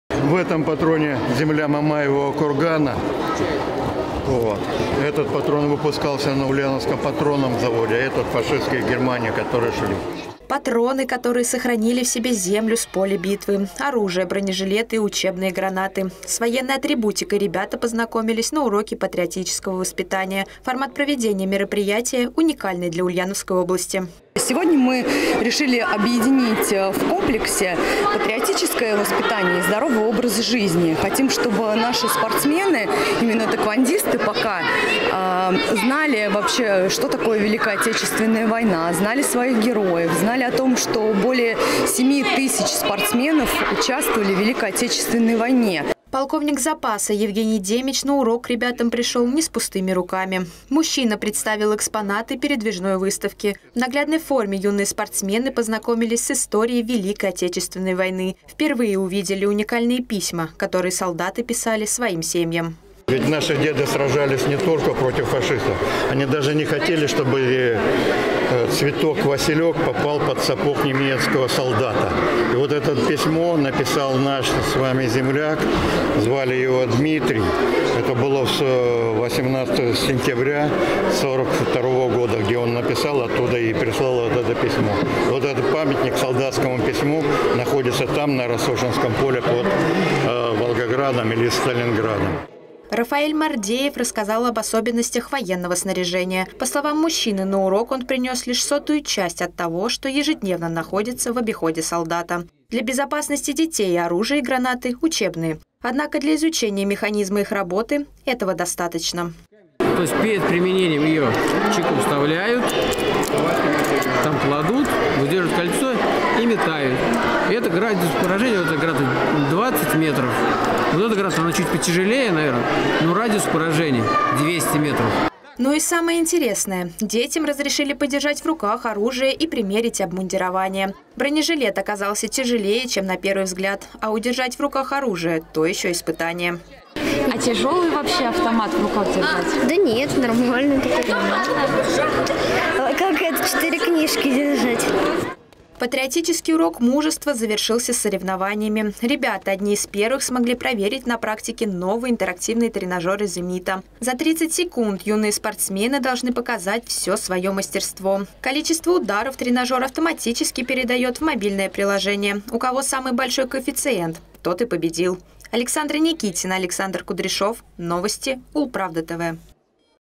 С любовью к отчизне. Как ульяновских спортсменов знакомят с историей
Патроны, которые сохранили в себе землю с поля битвы, оружие, бронежилеты и учебные гранаты. С военной атрибутикой ребята из спортшколы по тхэквондо познакомились на необычном уроке патриотического воспитания.